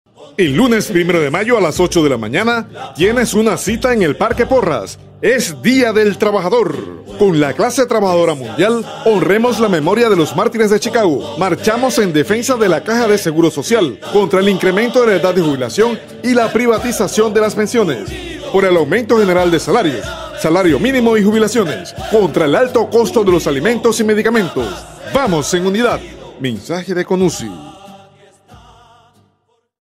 CONUSI : 1 de Mayo 2023 - Honramos la memoria de los Mártires de Chicago
Lunes 1 de mayo, 8: 00 A.M. / Parque Porras
Día internacional de los trabajadores y las trabajadoras
Honramos la memoria de los Mártires de Chicago

Marchamos en defensa de la caja del seguro social, contra el incremento de la edad de jubilación y la privatización de las pensiones, por aumento general de salario, salario mínimo y jubilaciones, contra el alto costo de los alimentos y medicamentos...

¡Bárbara Mejía Presente!